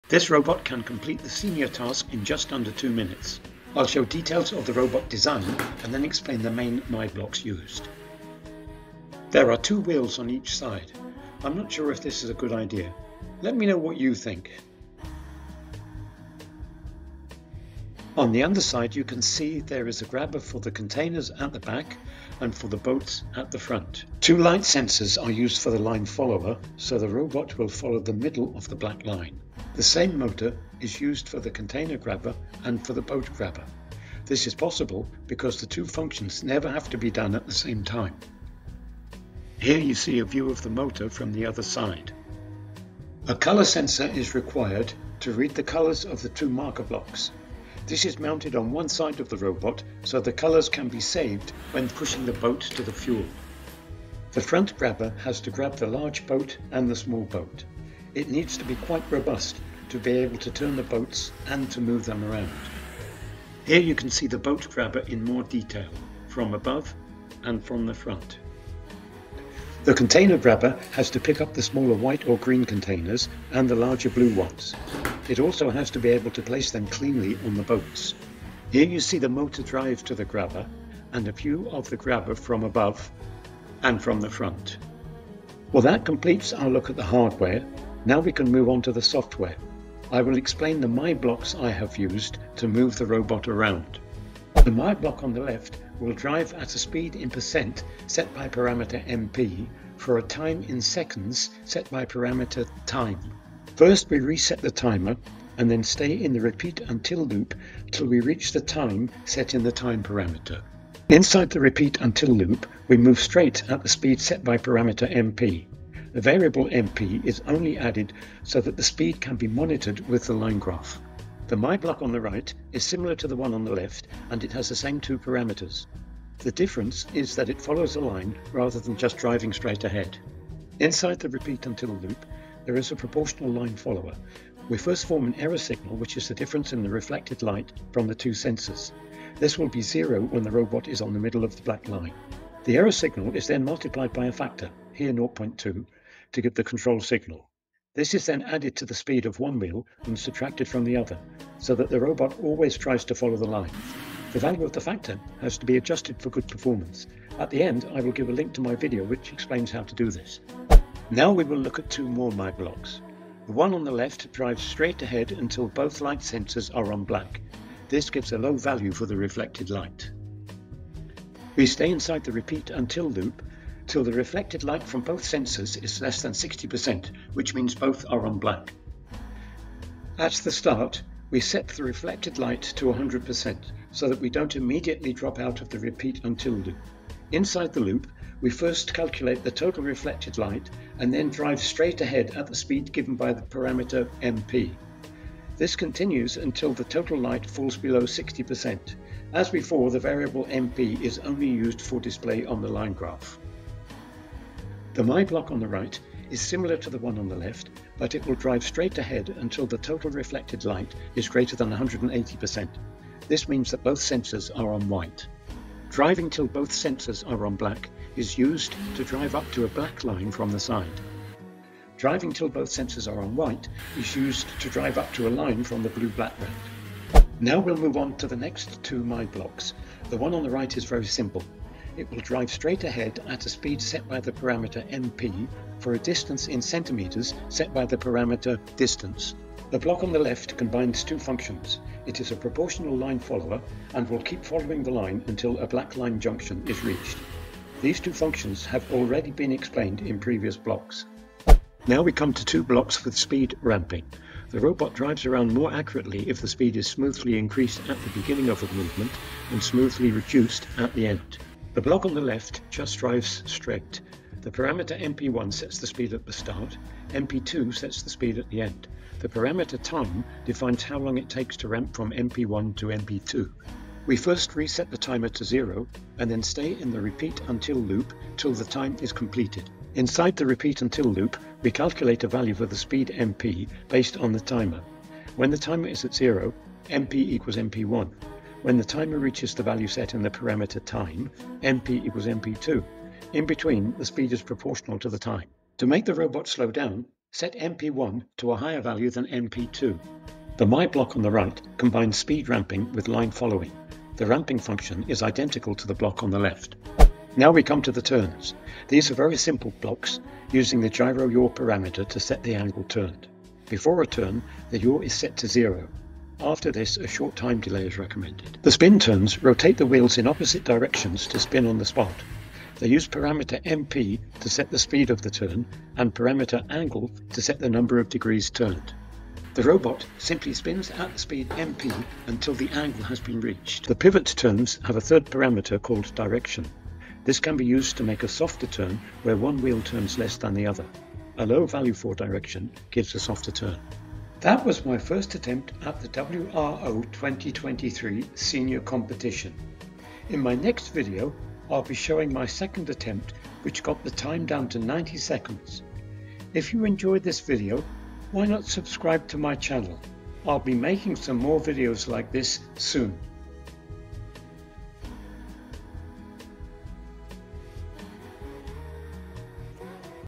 Details of WRO2023 2 Minute Spike Prime
This Spike Prime robot is a first attempt to beat the 2 minute deadline for WRO2023 Senior competition. Building and programming details are shown.  It is the first of three robots, each beating the previous time by 25%. A detailed explanation of the main MyBlocks used is given. Keep watching for details of the fast and super fast robots to come!